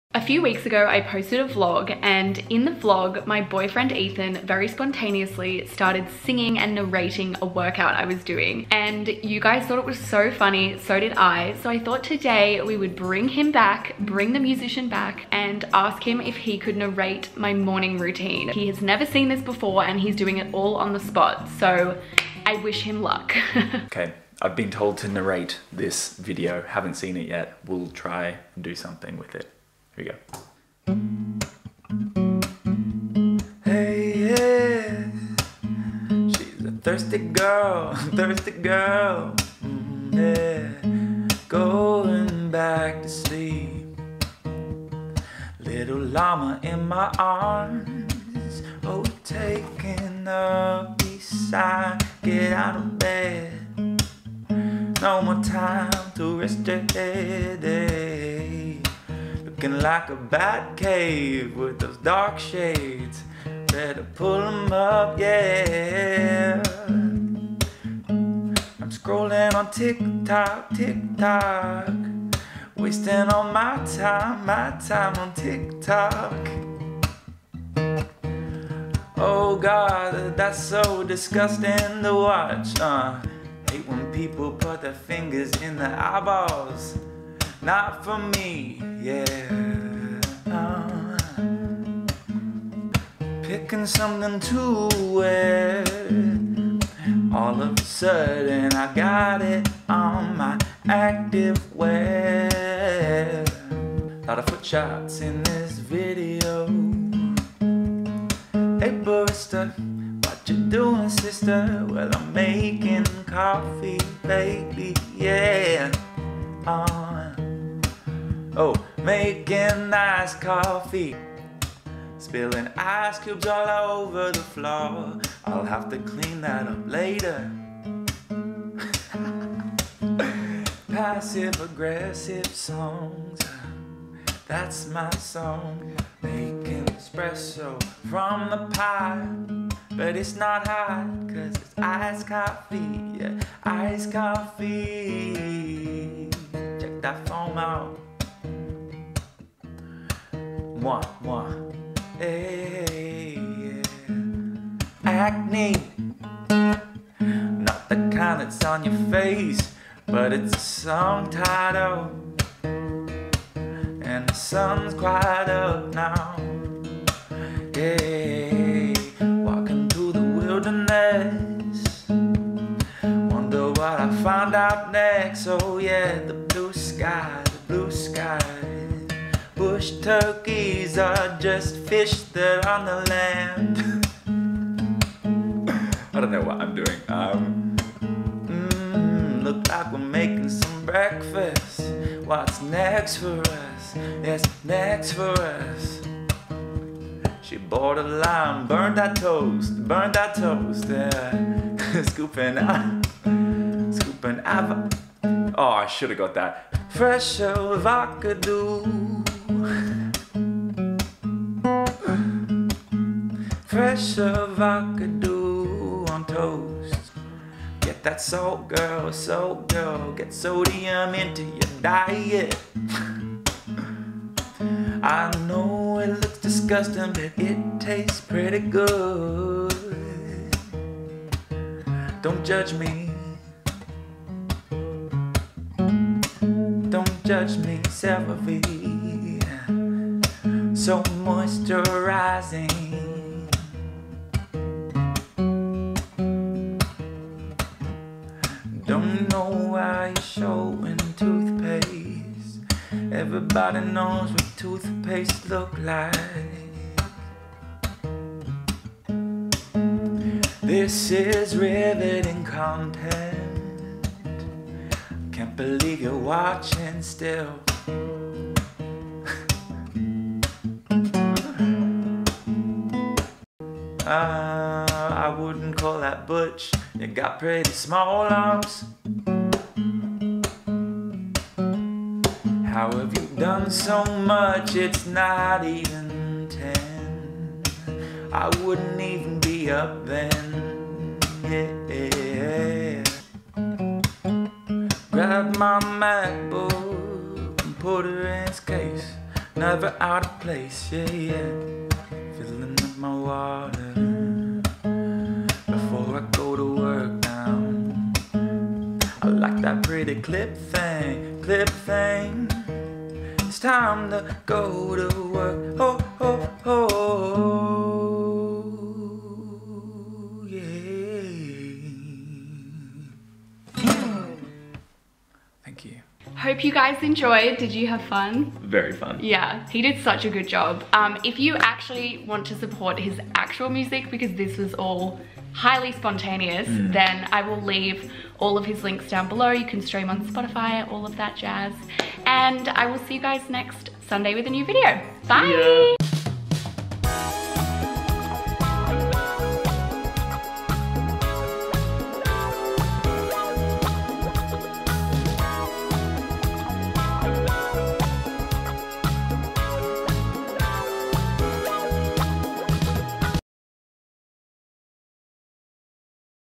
i made my musician bf voiceover my morning routine through✨song✨
hi my dudes! i asked my musician bf to voiceover my morning routine through song... it's absolutely iconic. comment ur favourite part down below lol.

love u guys sm